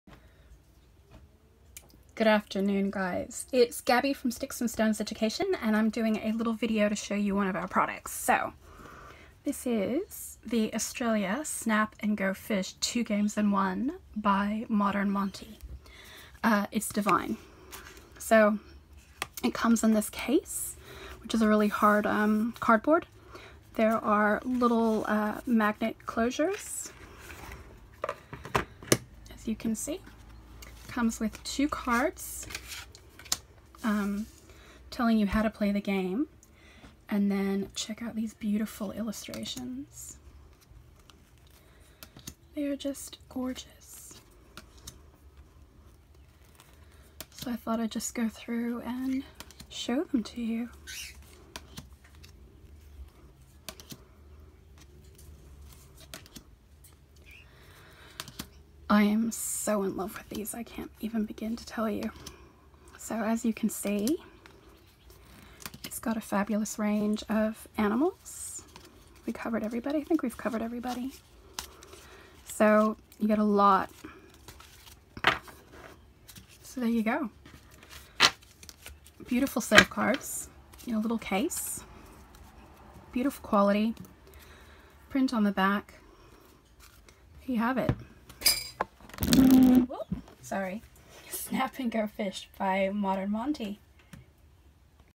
Australia Snap & Go Fish || Modern Monty
Australian Animal Snap & Go Fish Game is a  reimagined classic card game combining two games in one set of cards: Snap AND Go Fish packaged together in the most luxurious, magnetic-close box. This beautiful set is ethically made and eco-friendly.

The all original, watercolour art by Australian artist Amanda Borchers features the amazing native animals that live in Australia. The art will prompt conversation and learning about Australian wildlife, while your little one will also learn skills to take turns, utilise strategy in game play, match pairs, utilise simple math, listening and memory skills and of course learn good sportsmanship.

There are four cards of each Australian animal character:
Bilby
Kookaburra
Little penguin
Sulfur-crested cockatoo
Australian fur seal
Sugar glider
Platypus
Crocodile
Emu
Koala
Kangaroo
Wombat
Echidna

SPECIFICATIONS:

Cards are made from premium, durable, 900gsm, recycled cardstock.
Cards come in a cardboard box with a magnetic lid.
Our manufacturer is FSC certified, which means our product is eco-friendly and ethically made. It is made with environmentally and socially responsible processes.
Designed and illustrated in Australia, manufactured over seas.
How to Play?
Game instructions included:
1) Simple Snap (printed on the back of the box)
2) Regular Snap (enclosed in the box)
3) Go Fish (enclosed in the box)